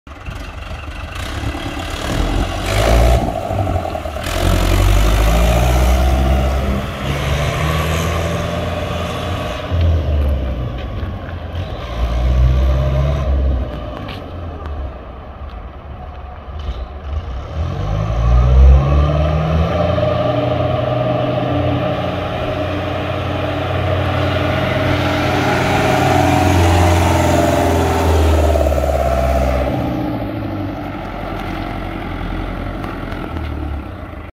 BV16821 Fiat 780DT
Fiat 780DT
4x4
Good tires
Tractor is in good working condition

Stock nr: BV16821